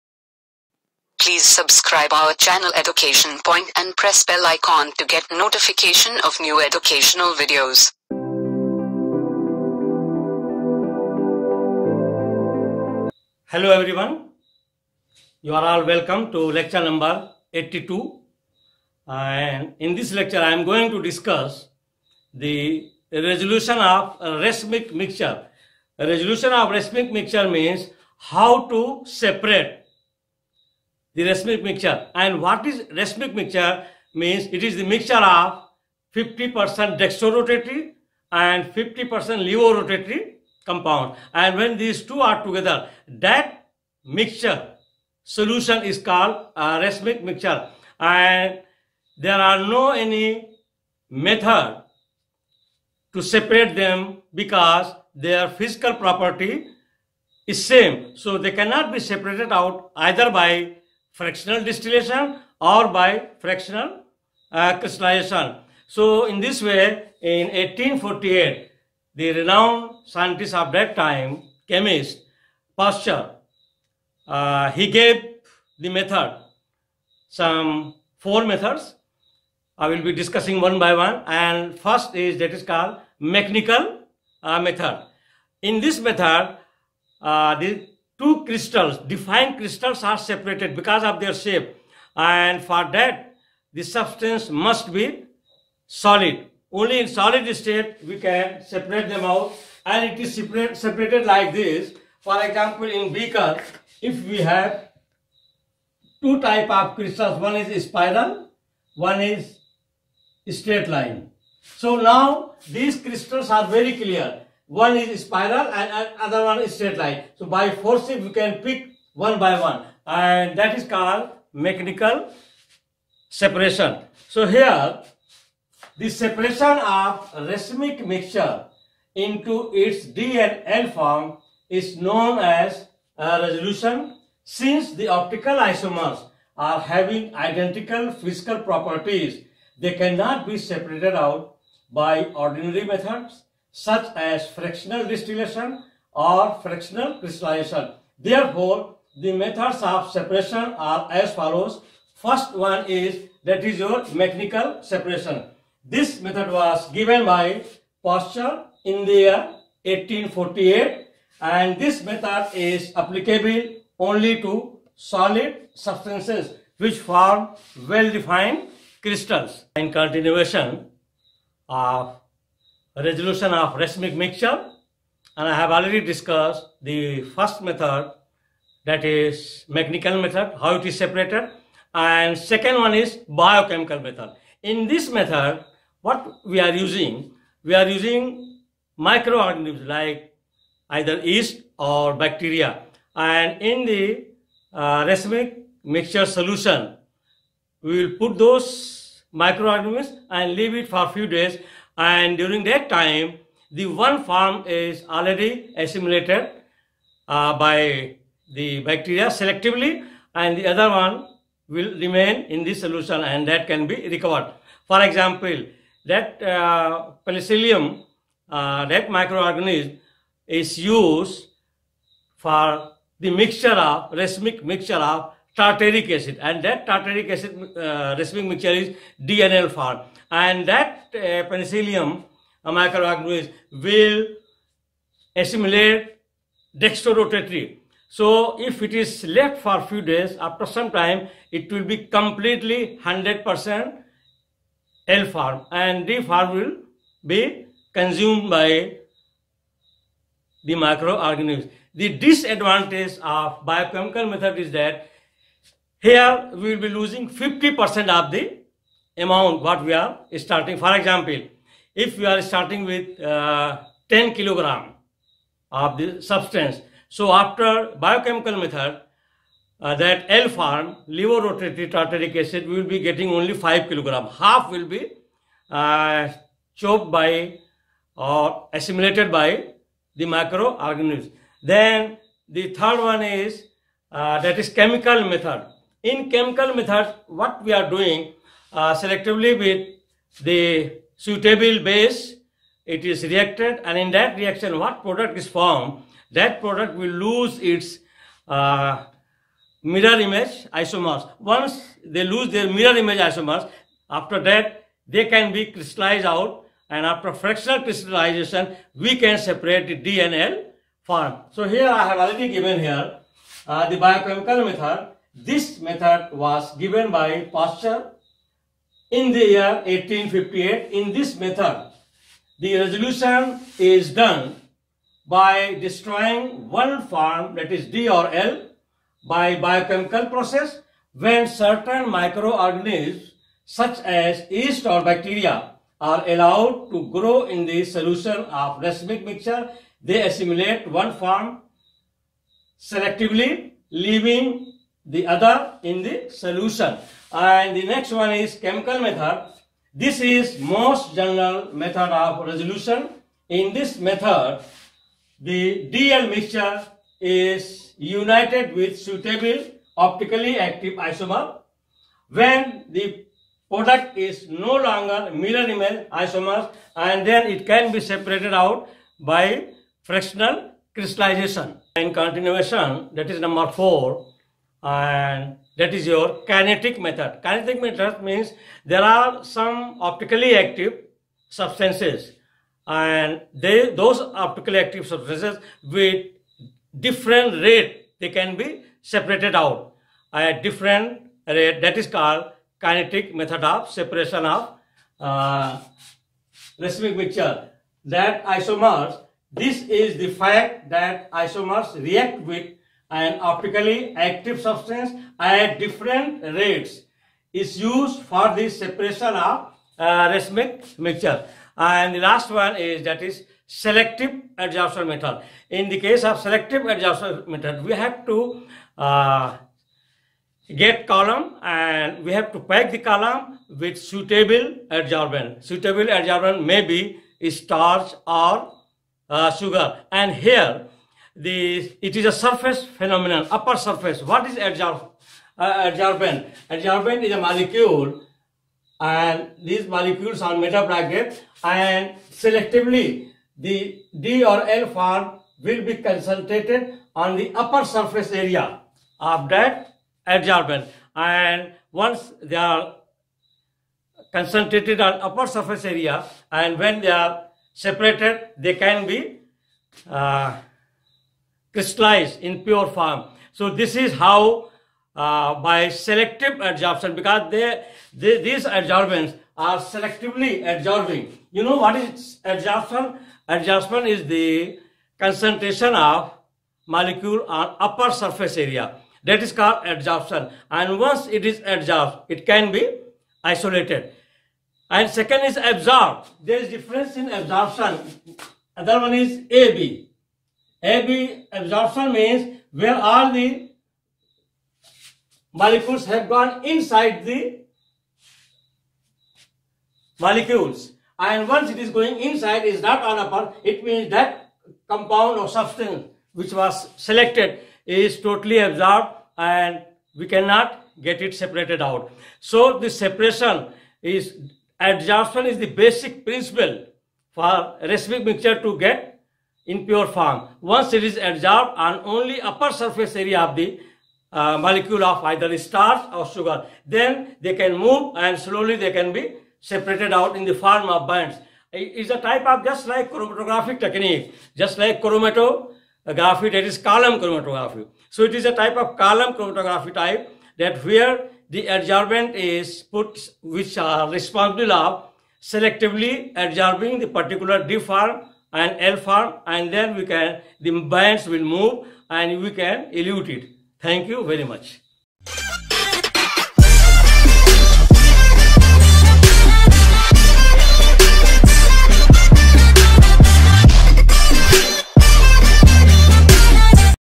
General Chemistry Lecture 82 - Resolution of Racemic Mixture
The separation of a racemic mixture into it's d and l form is termed resolution. Since the optical isomers are having identical physical properties they can not be separated out by ordinary methods such as fractional crystallization and fractional distillation. So the other methods of separation is given here.
Title: General Chemistry Lecture 82 - Resolution of Racemic Mixture
Instructor: Prof. I. Z. Khan